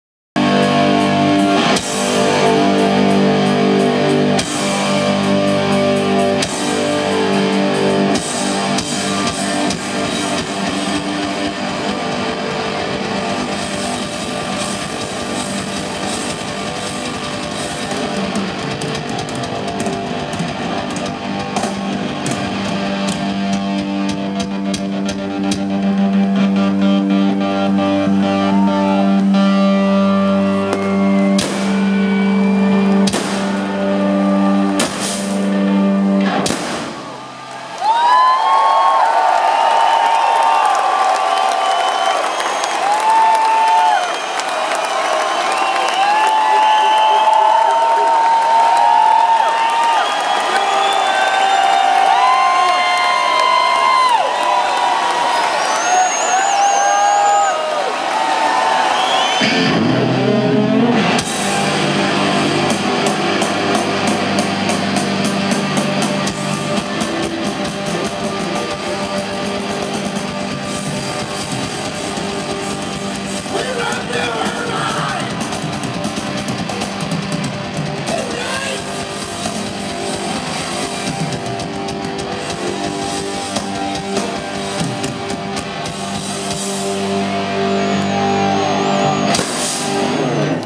KISS drummer Eric Singer rises above stage Verizon Wireless Amphitheatre, Irvine, CA 8-14-12.MTS
KISS Eric Singer, drummer rises on platform above stage.Verizon Wireless Amphitheater, Irvine, CA 8-14-12